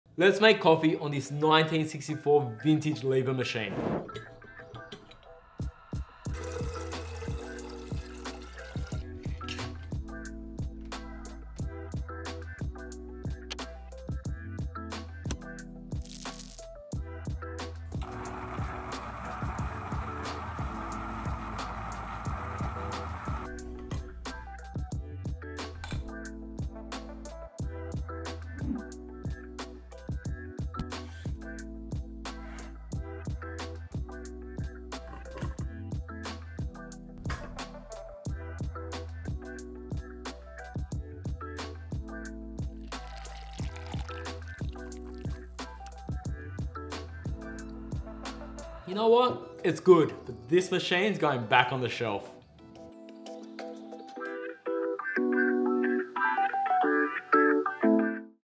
Making an espresso on this 58 year old espresso machine!
Making an espresso on this 58 year old espresso machine! 1964 Faemina 2nd Model "Blue Label" Rebuild

Full video coming out soon!

Find Coffee Parts on Socials